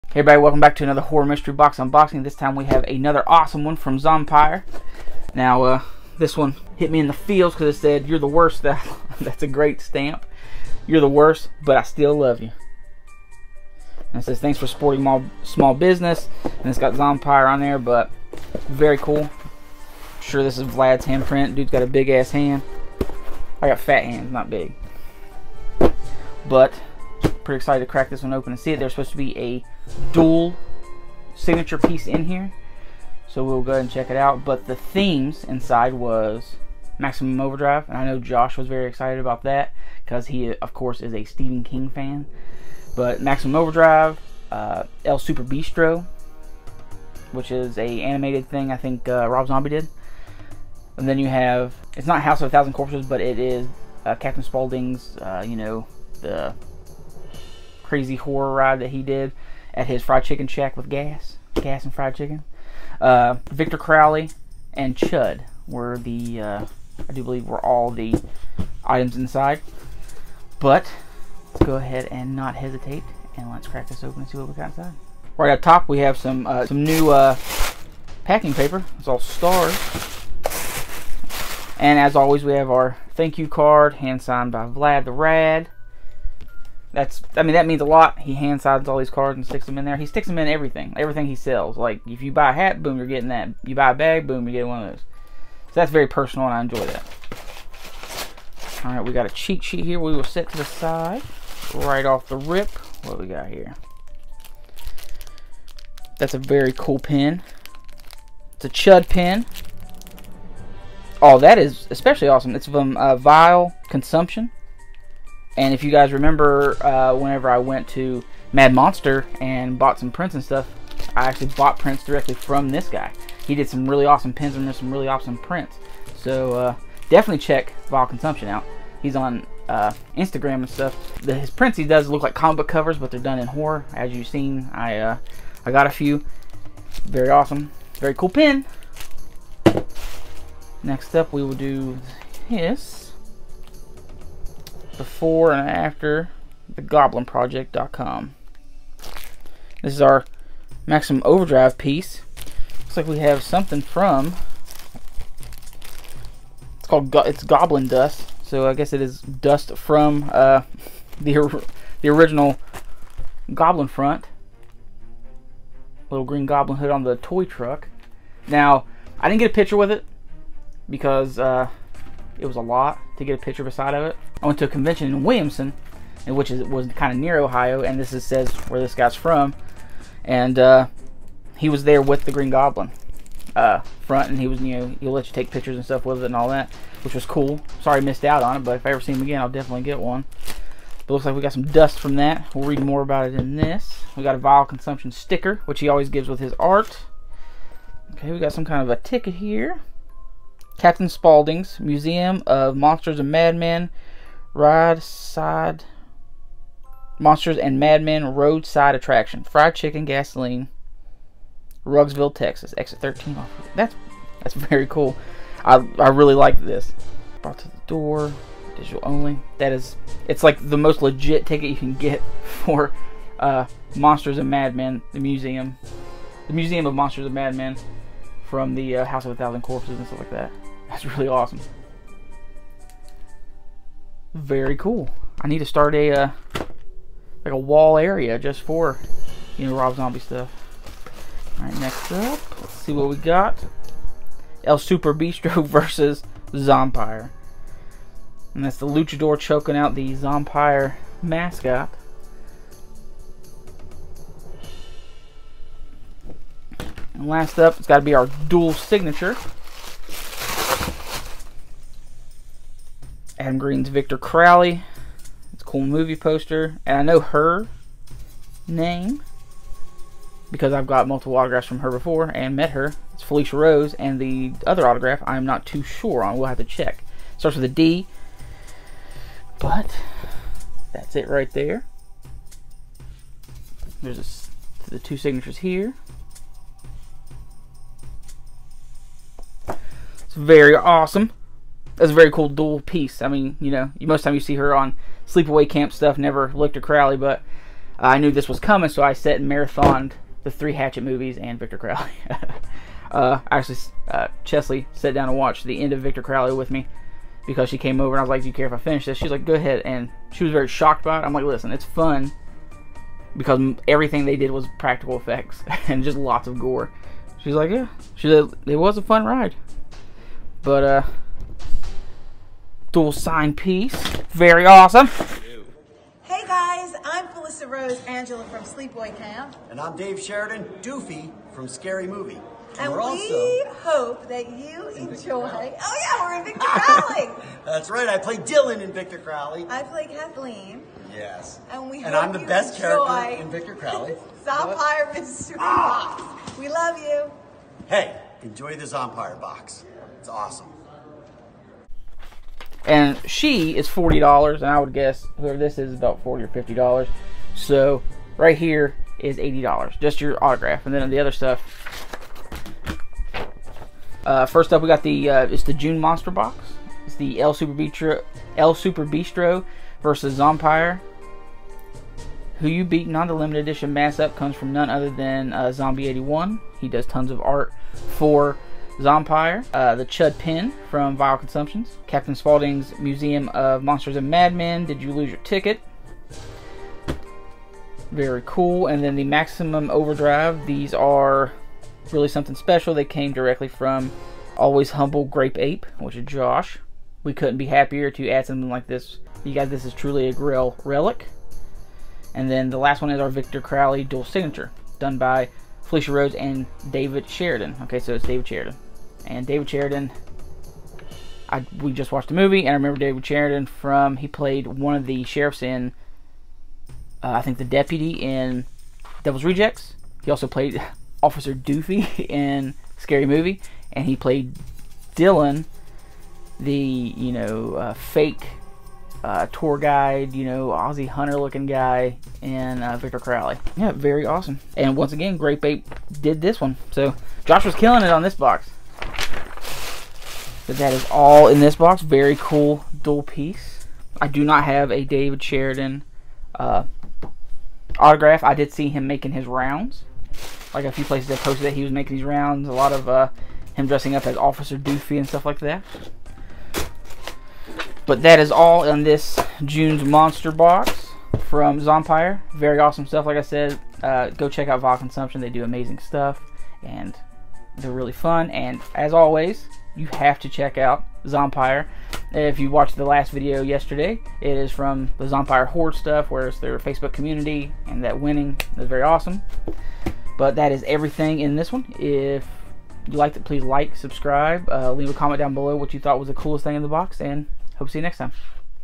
NEW!! Zompire's Monster Box (June 2021)
Hey everyone lets check out the April's monster box from Zompire. This is a bi-monthly box from these guys and the themes inside were

Captain Spauldings Museum of Monsters and Madmen
Victor Crowley
Maximum Overdrive
El Superbeasto